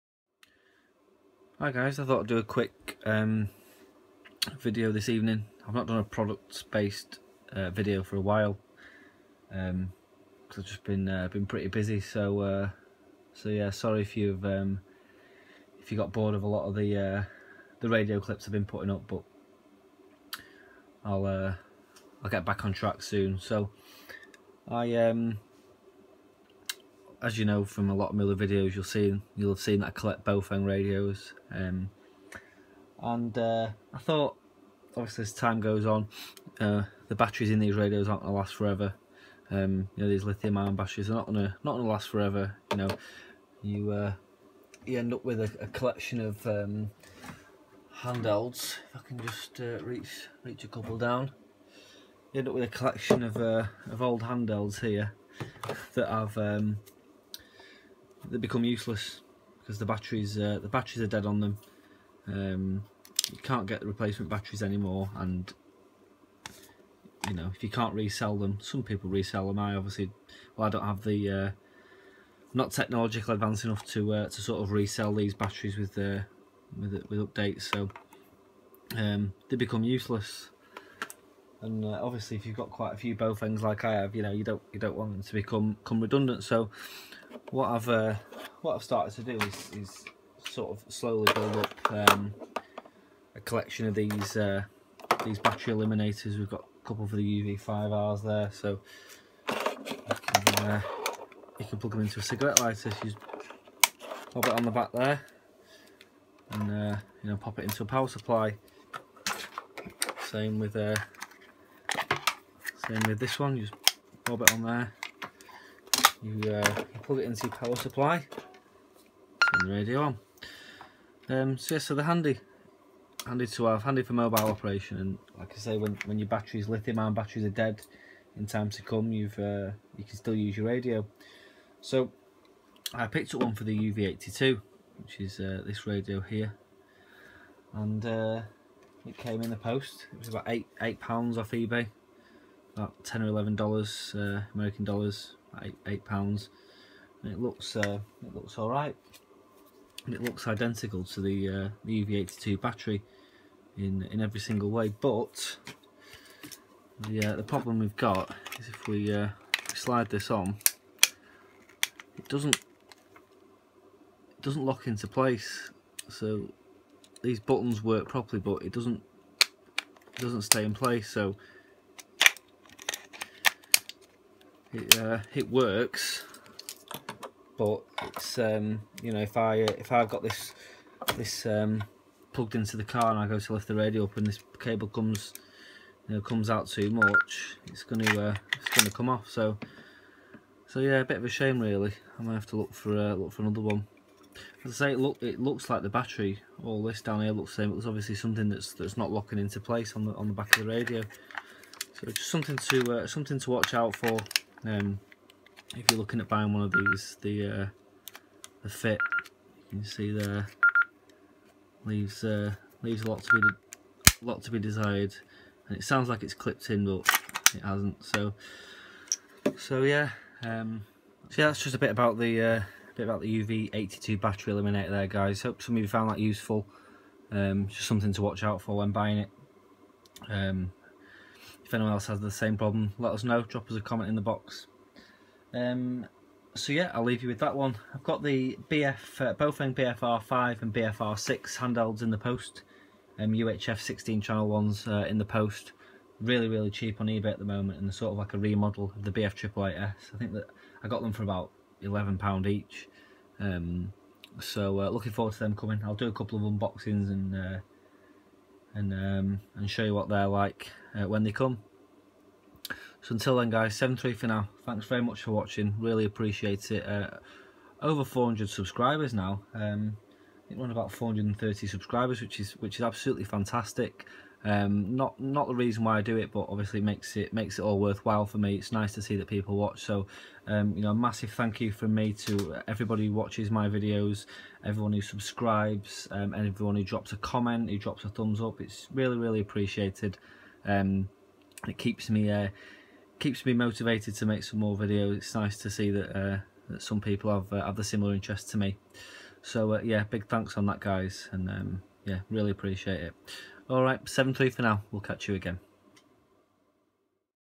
Baofeng UV-82 Battery Eliminator Issue | 420+ Subs Thanks!
UV-82 battery eliminator doesn't fit properly!